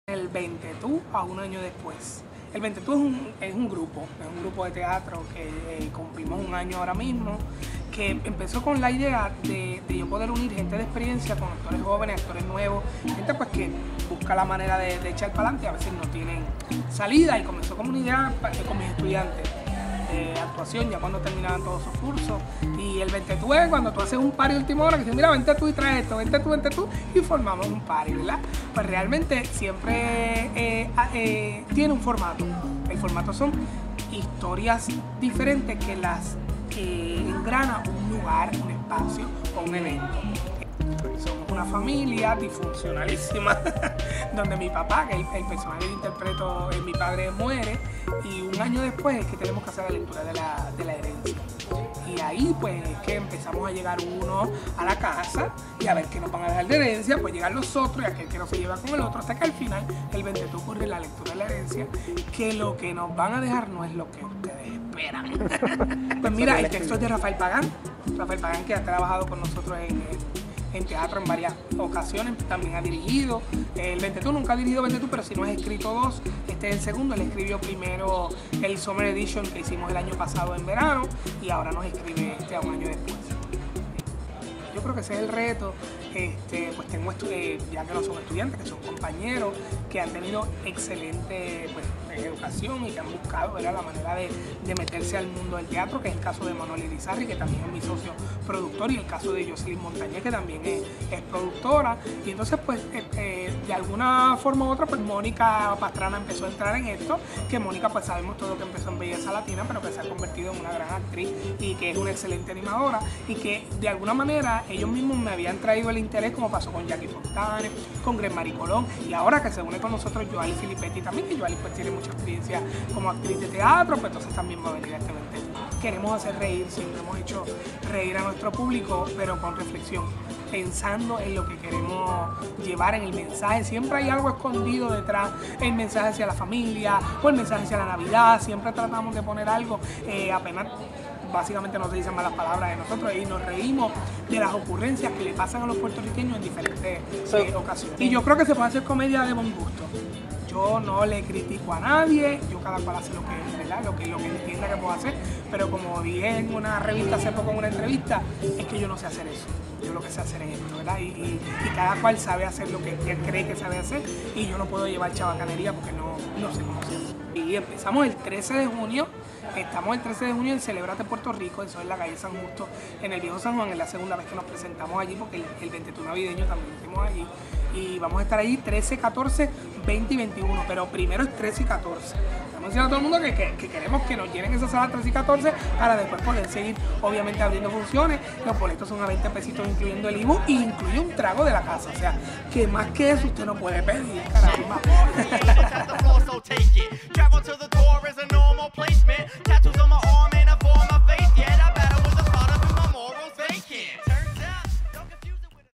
EL VENTETÚ / Munglo.tk
Àngel Manuel llega a nuestra mungloteca para hablarnos de la nueva obra del Ventetú. En esta nueva puesta el grupo se centra en la repartición de una herencia. Esto será el eje de esta nueva comedia en El Ventetú a un año después los días, 13, 14, 20 y 21 de junio en Celebrate Puerto Rico en el Viejo San Juan a las 8:30 pm.